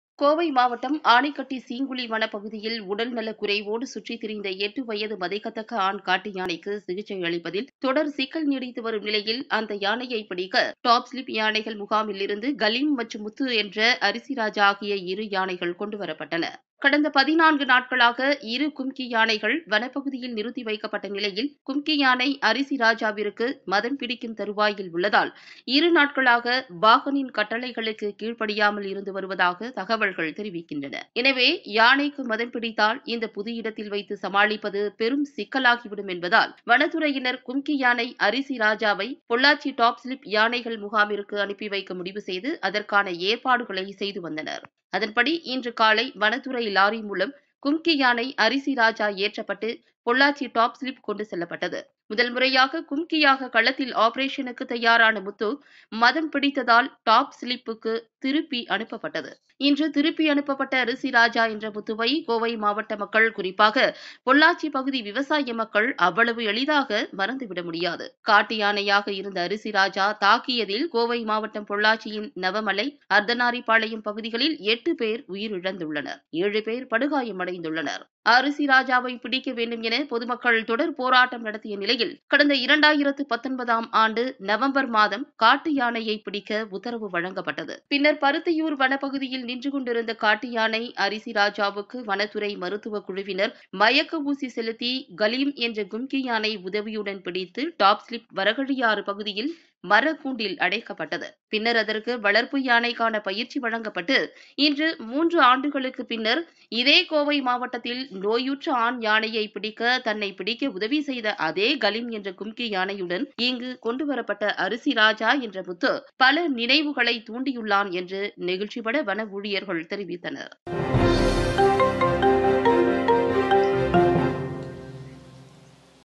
"முத்து" என்ற கும்கி யானைக்கு மதம் பிடித்ததால் திரும்ப அனுப்ப வைப்பு
Kumki Muthu alias Arisi Raja transported back to Topslip due to musth condition.